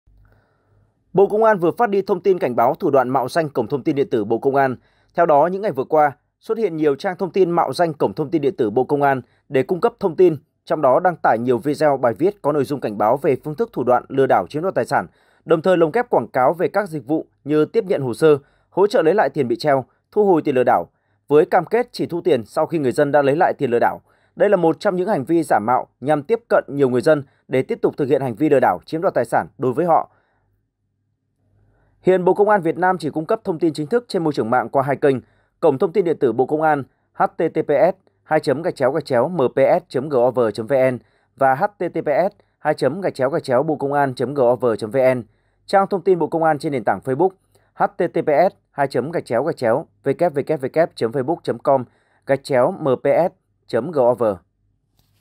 Cảnh báo thủ đoạn mạo danh Cổng Thông tin điện tử Bộ Công an
Hưng Yên TV | Bộ Công an vừa phát đi thông tin cảnh báo thủ đoạn mạo danh Cổng Thông tin điện tử Bộ Công an.
Theo đó, những ngày vừa qua xuất hiện nhiều Trang Thông tin mạo danh Cổng Thông tin điện tử Bộ Công an để cung cấp thông tin, trong đó đăng tải nhiều video, bài viết có nội dung cảnh báo về phương thức, thủ đoạn lừa đảo chiếm đoạt tài sản; đồng thời lồng ghép quảng cáo về các dịch vụ, như: “tiếp nhận hồ sơ”, “hỗ trợ lấy lại tiền bị treo”, “thu hồi tiền lừa đảo”... với cam kết chỉ thu tiền sau khi người dân đã lấy lại tiền lừa đảo.
Đây là một trong những hành vi giả mạo, nhằm tiếp cận người dân để tiếp tục thực hiện hành vi lừa đảo, chiếm đoạt tài sản đối với họ. Hiện Bộ Công an Việt Nam chỉ cung cấp thông tin chính thức trên môi trường mạng qua hai kênh: Cổng Thông tin điện tử Bộ Công an và Trang Thông tin Bộ Công an trên nền tảng Facebook
Địa chỉ: 164 Nguyễn Văn Linh, thành phố Hưng Yên, tỉnh Hưng Yên
Tiktok: Truyền hình Hưng Yên
Facebook: Đài Phát thanh và Truyền hình Hưng Yên
Zalo: Đài PTTH Hưng Yên